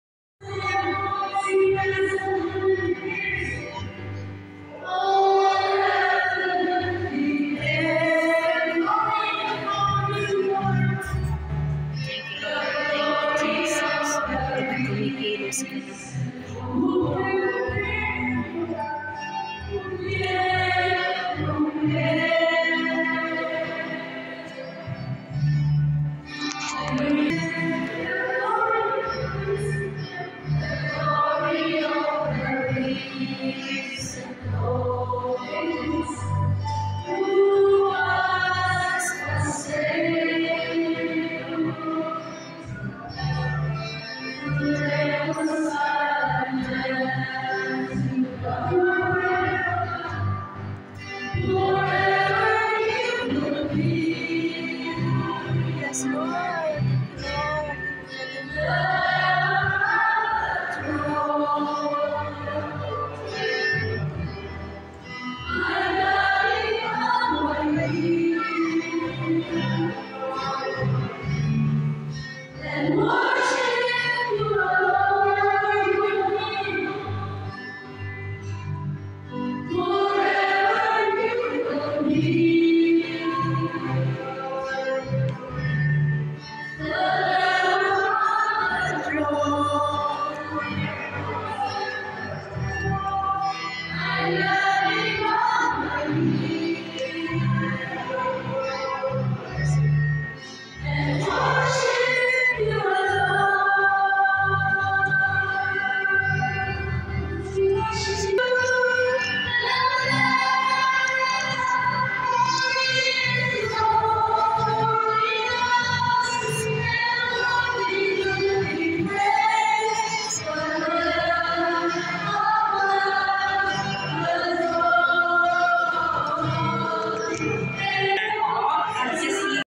10 Thanksgiving anniversary El Shaddai Windsor Prayer Partners. May 13/23.
Congratulations! God bless us all!
Brothers and sisters in Christ here in Windsor Outreach. To God be the glory!